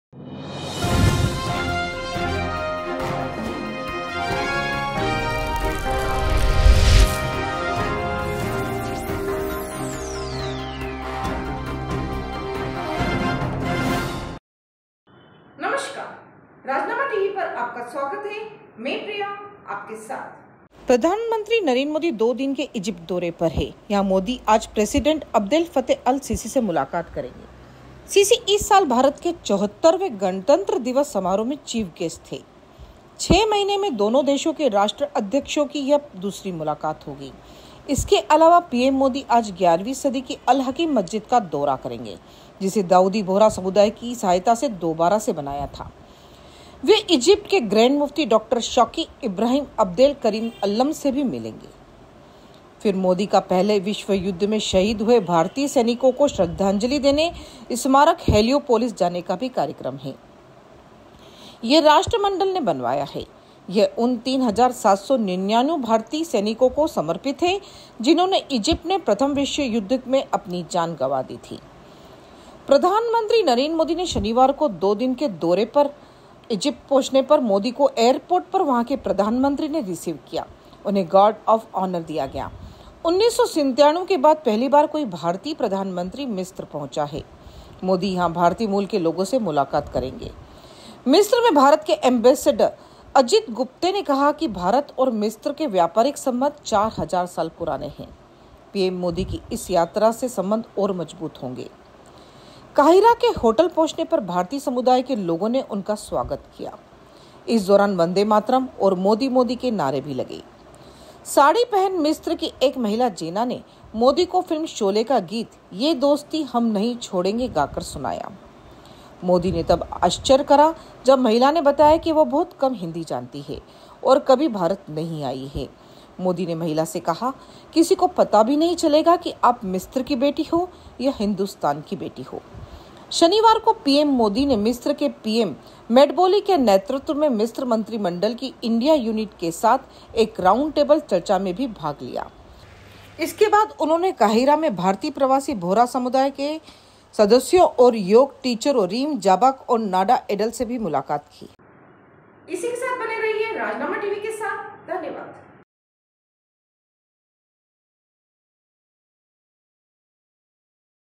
anchorpriya मोदी आज इजिप्ट के प्रेसिडेंट से मिलेंगे
About this video:मोदी आज इजिप्ट के प्रेसिडेंट से मिलेंगे